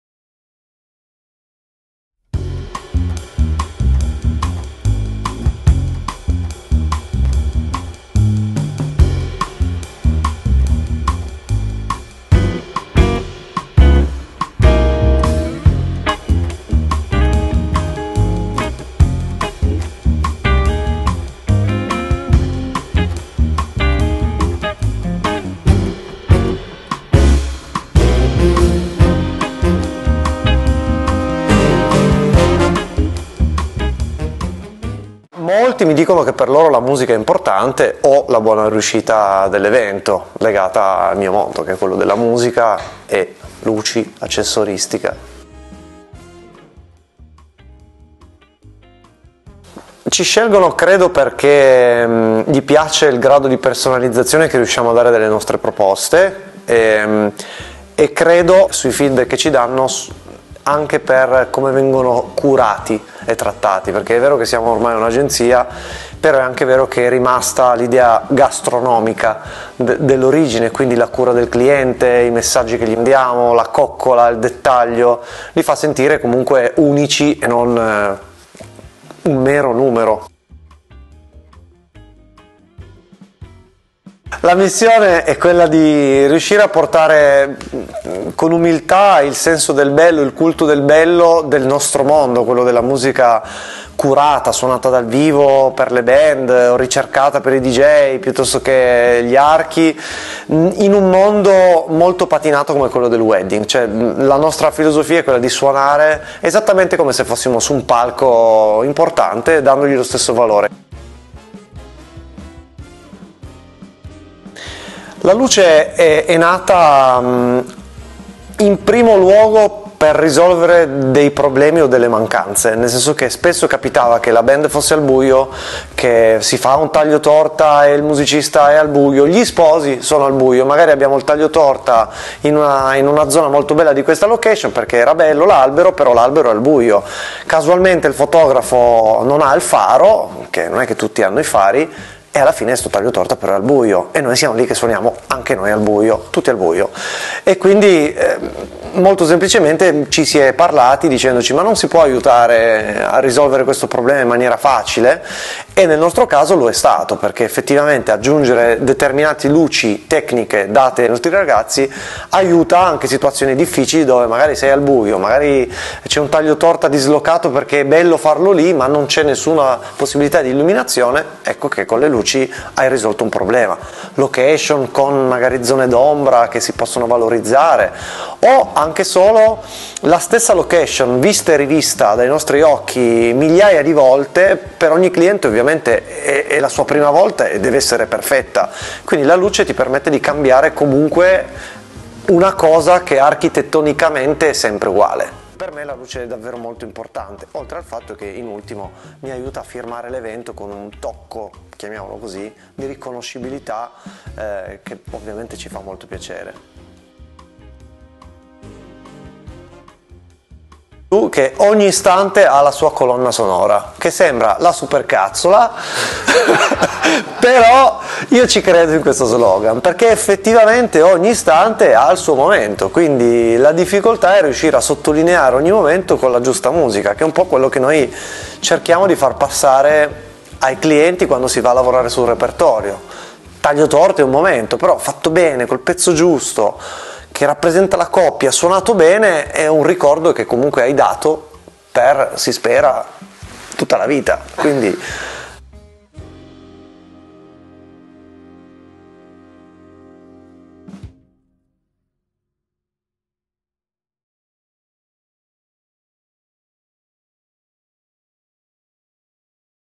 Daniele Pavignano - Musica e illuminazione per un matrimonio unico
In questa intervista Daniele Pavignano, un musicista specializzato in matrimoni, rivela il suo segreto per avere successo: personalizzare la sua musica e prestare attenzione ai dettagli. Nonostante sia un'agenzia, Pavignano vuole far sentire i suoi clienti speciali e unici, creando un'esperienza musicale personalizzata che sia perfetta per ogni momento dell'evento.

Ma non è solo la musica ad essere importante per Pavignano - ha anche introdotto l'illuminazione come parte del suo servizio, poiché ha notato che può fare la differenza in un'occasione speciale come il matrimonio. La luce può creare un'atmosfera magica, valorizzare le zone d'ombra e dare un tocco di riconoscibilità all'evento.

Per Pavignano, ogni momento dell'evento merita la sua colonna sonora unica, compreso il taglio della torta. Il suo obiettivo è creare ricordi indimenticabili per i suoi clienti, in modo che possano rivivere quei momenti speciali per tutta la vita.